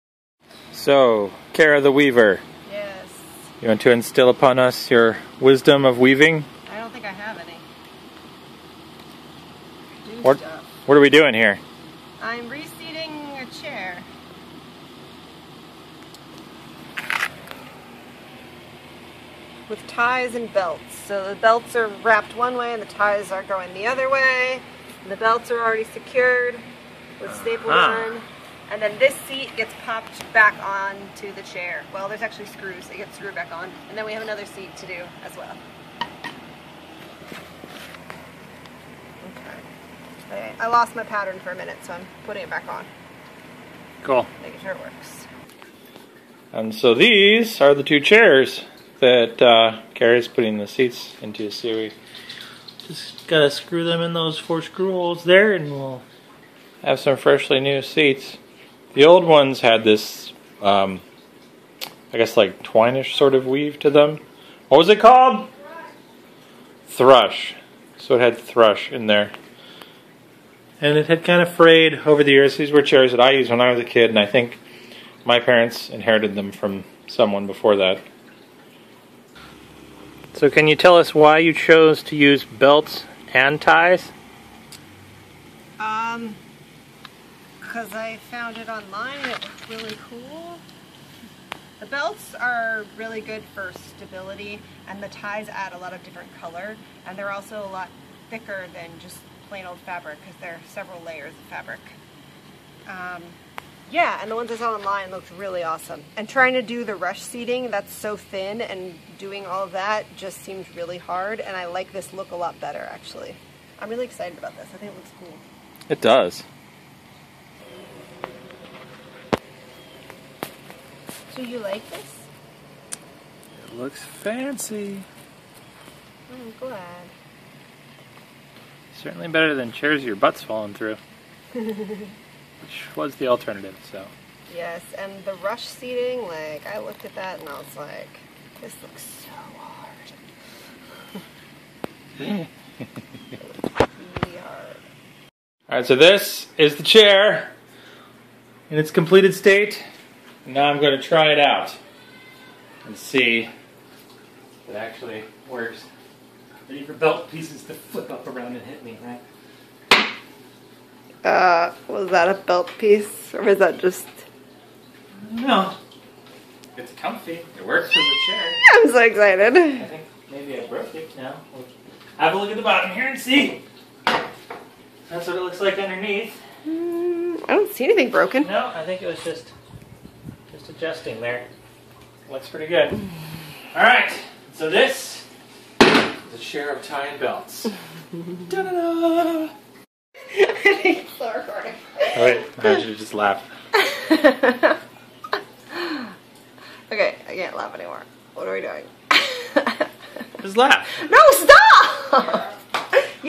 How To Reseat A Chair With Belts & Ties
We had an old chair where the rush seat was frayed & broken so we picked up some old ties & belts at Goodwill and made a new seat!